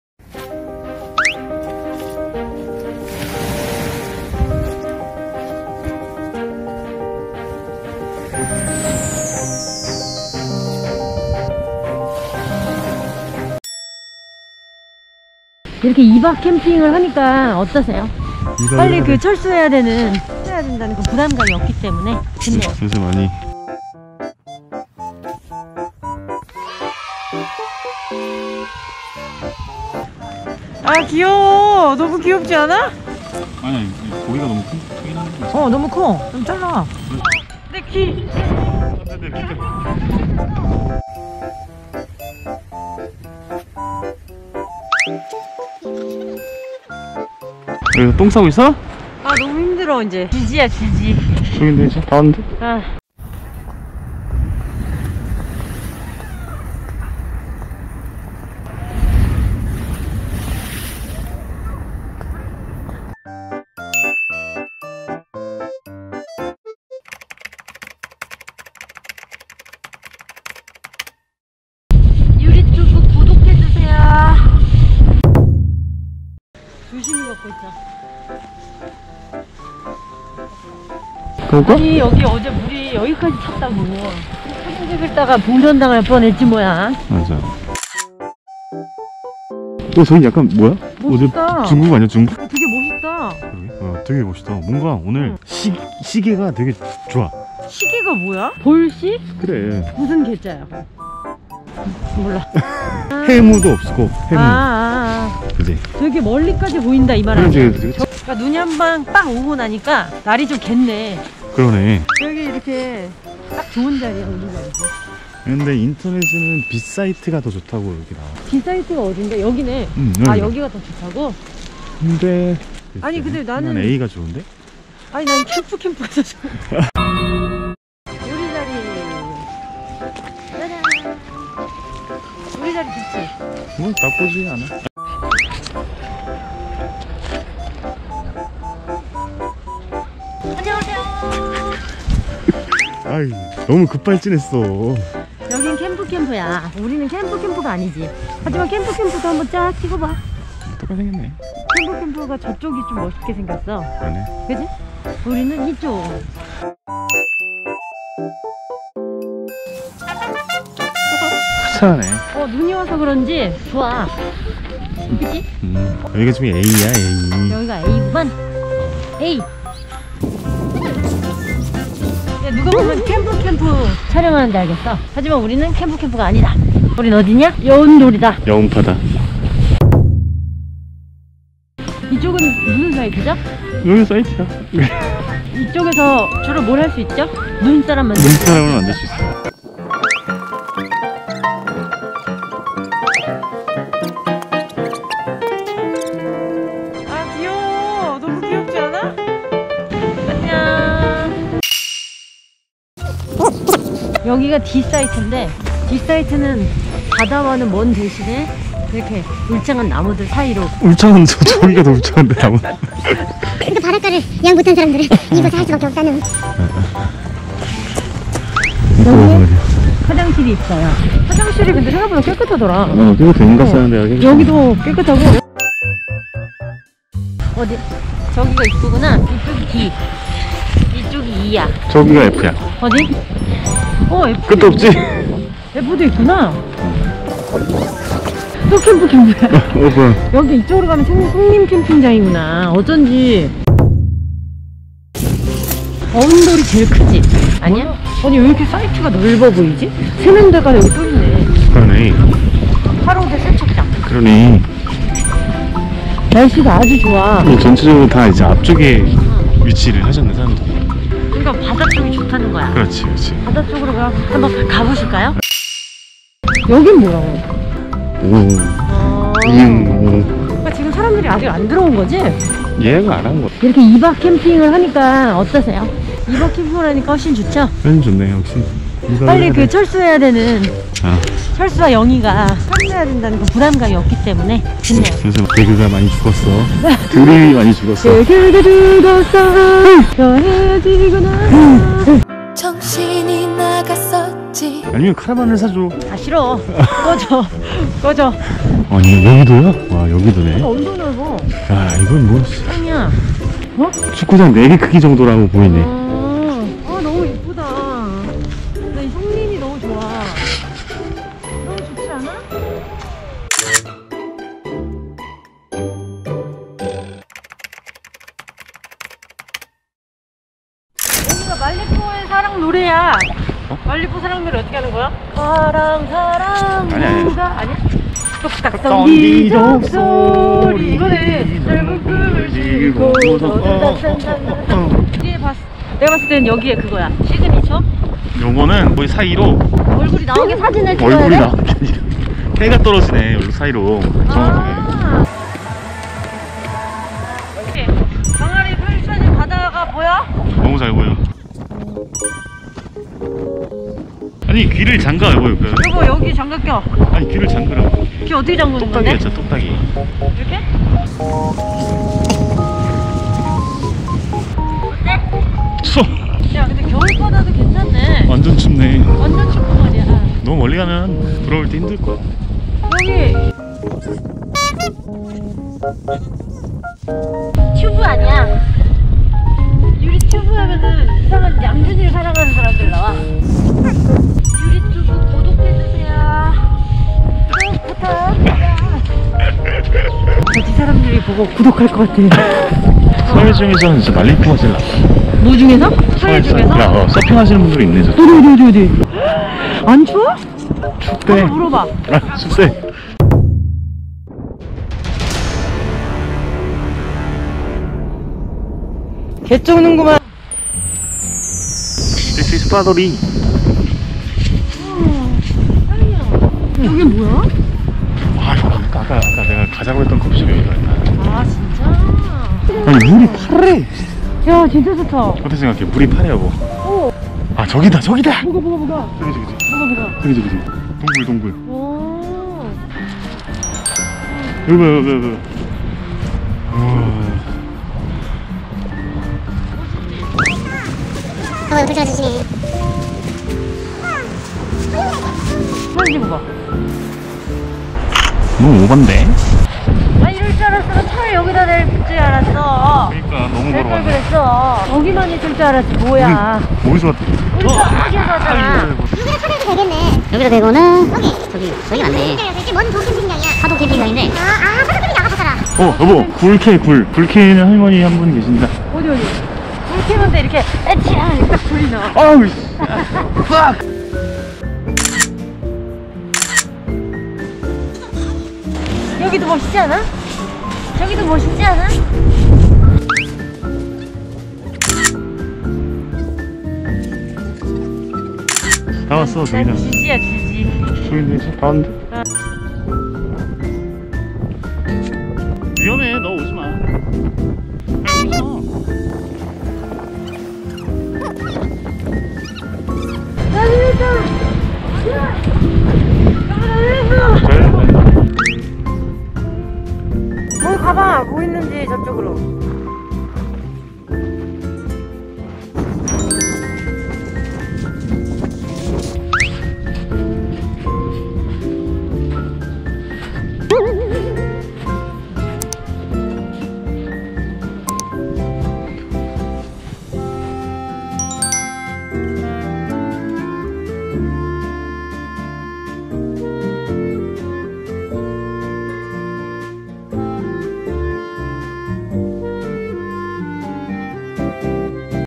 🧡어은돌 오토캠핑장 상세소개! 🧡이보다 자세할수 없다! 주변💙 만리포해안💙과 파도리 해식동굴 소개💕
안녕하세요 유리투브에요
오랜만에 인사드립니다!
태안에 있는
"어은돌 오토캠핑장"에 다녀왔어요
2박캠핑이었기떄문에
주변에 만리포해안과 유명한곳인 파도리해식동굴도 다녀왔으니